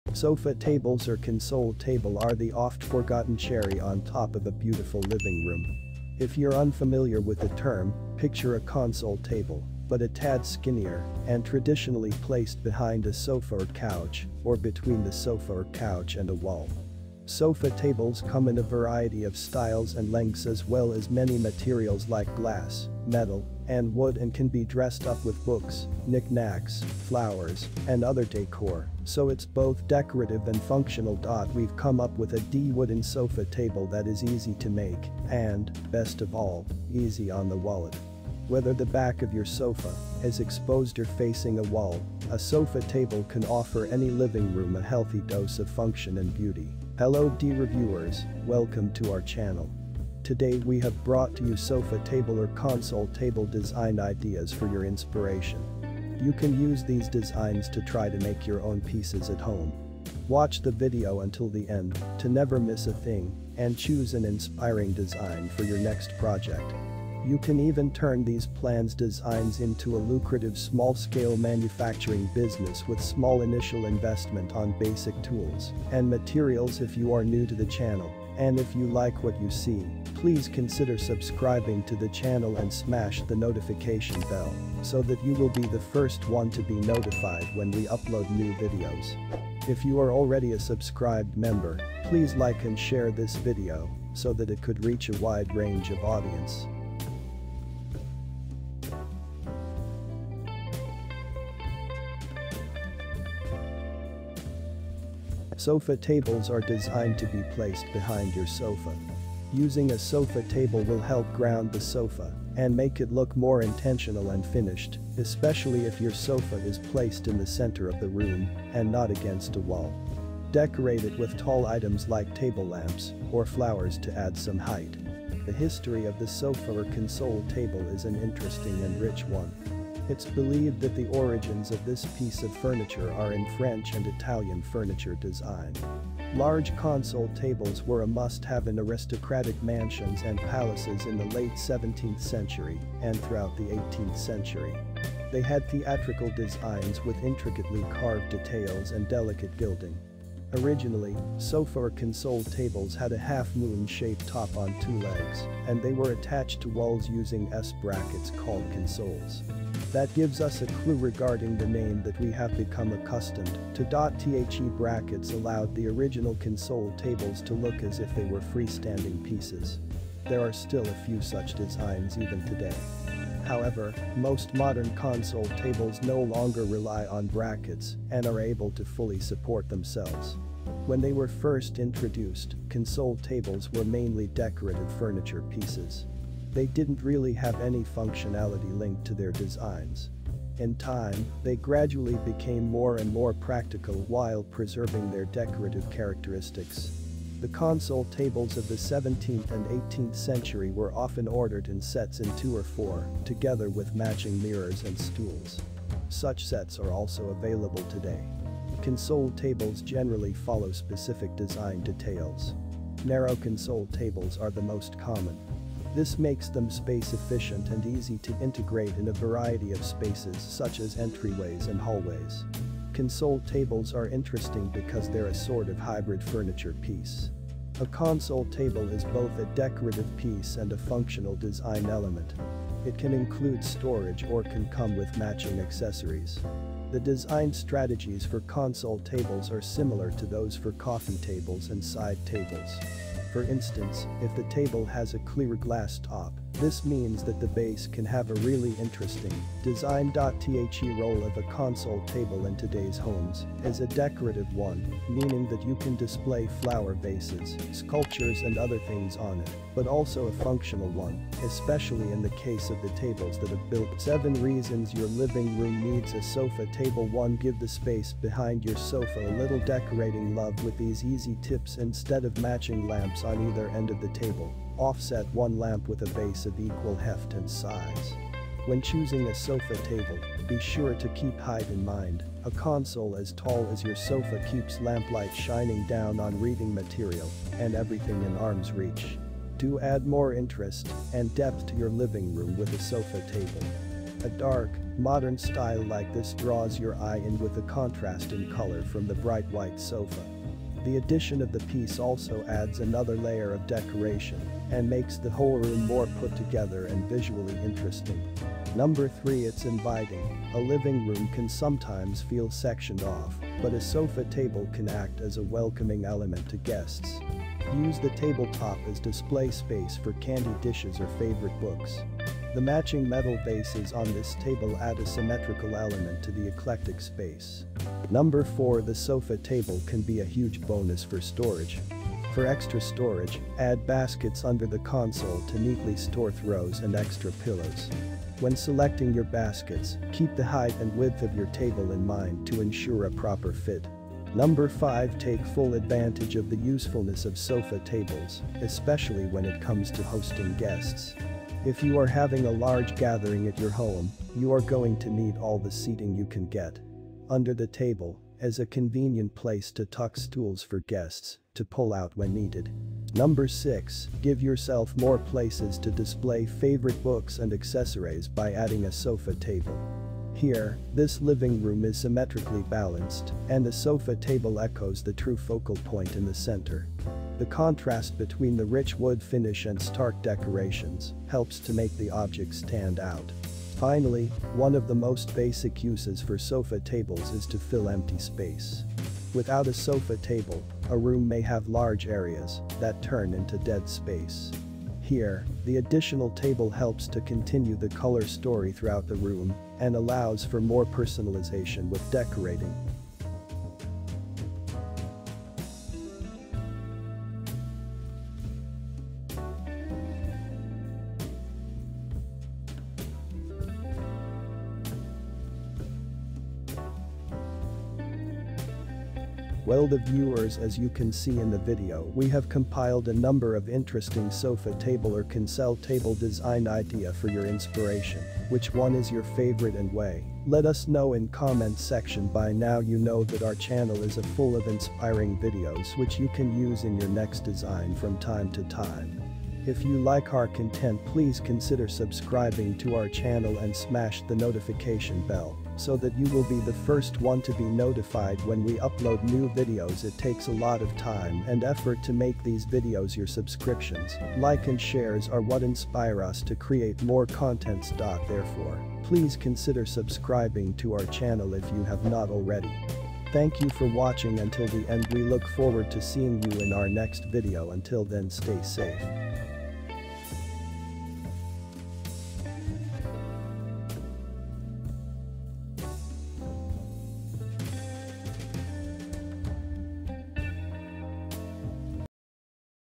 Creative Console Table design Ideas/Sofa Table / Woodworking project ideas/Coffee table/Dining table
Today we are here to show you a collection of Creative Console Table design Ideas. So, watch the video until the end to see these beautiful ideas and get inspiration.